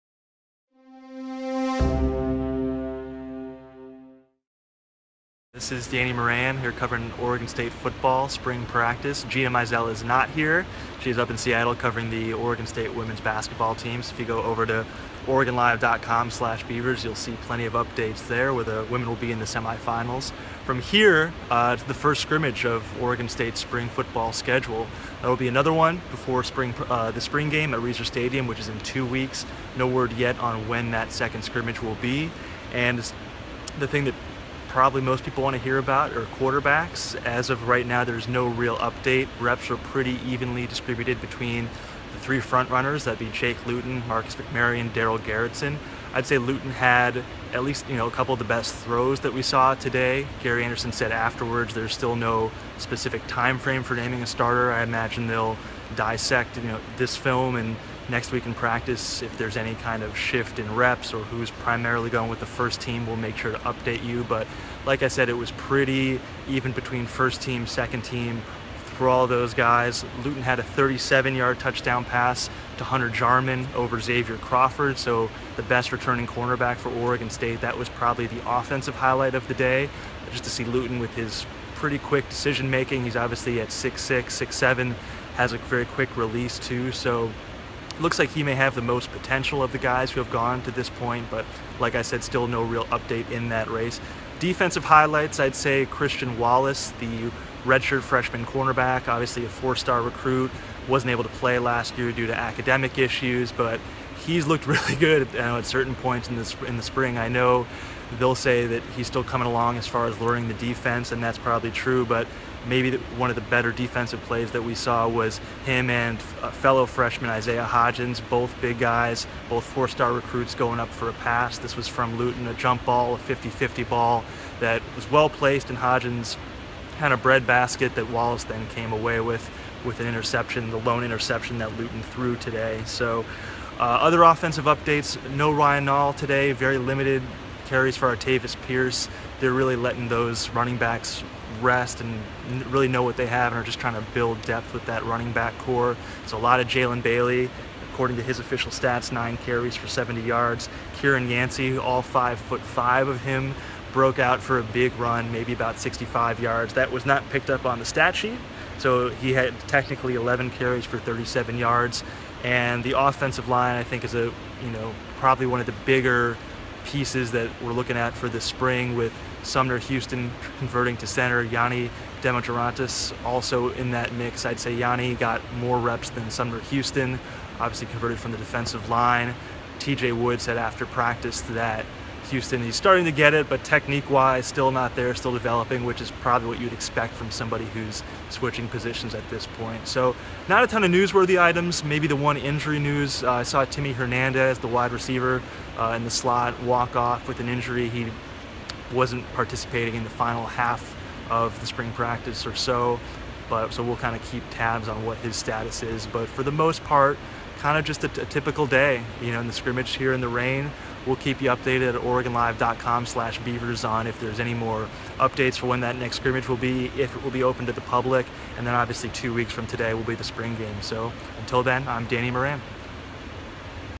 Oregon State Beavers Football Spring scrimmage report
The Oregonian/OregonLive Beavers beat reporter Danny Moran after OSU scrimmage on Saturday.